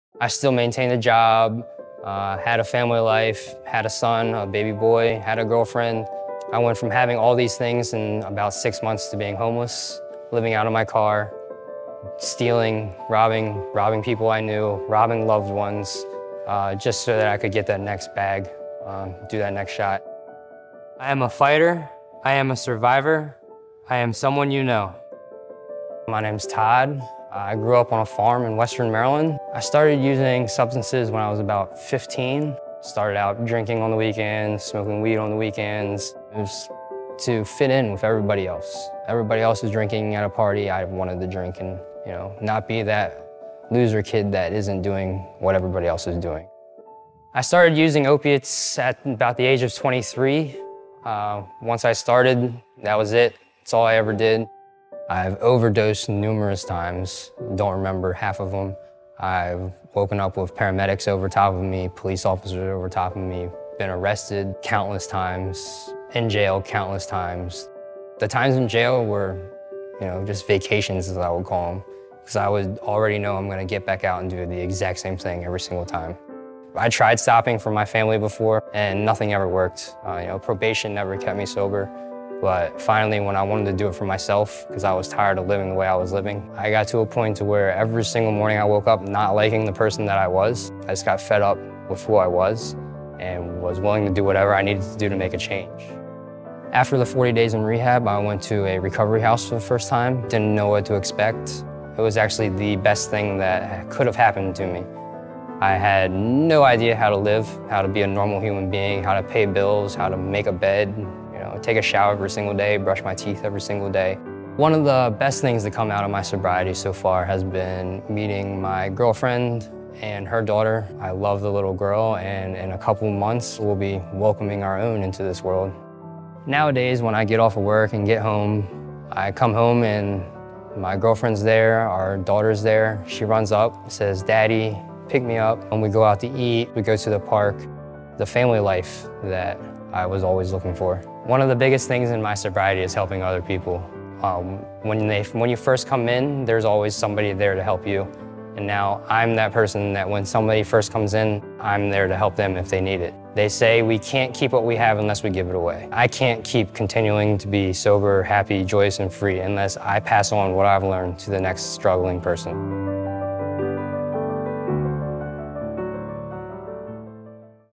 Someone You Know: Todd
Meet Todd, one of 23 million Americans who have overcome opioid use disorder, who wants you to know that recovery is possible. Now you know one more person affected by the opioid epidemic.

The Independence Blue Cross Foundation is working with the local community to remove the stigmas surrounding Opiate Use Disorder (OUD). Someone You Know is a project that shares personal stories from parents, children, friends, and loved ones affected by OUD.